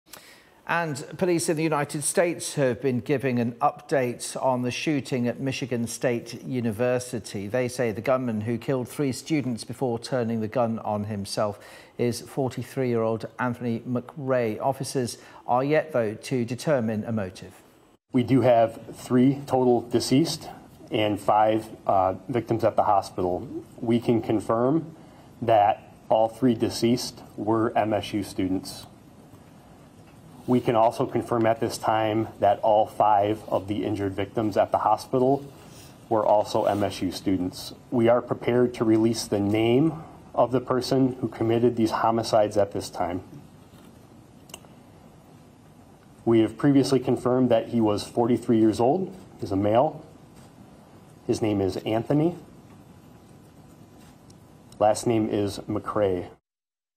Michigan State University gunman identified as 43-year-old Anthony McRae
Police say the gunman who killed himself after fatally shooting three students at Michigan State University was 43-year-old Anthony McRae.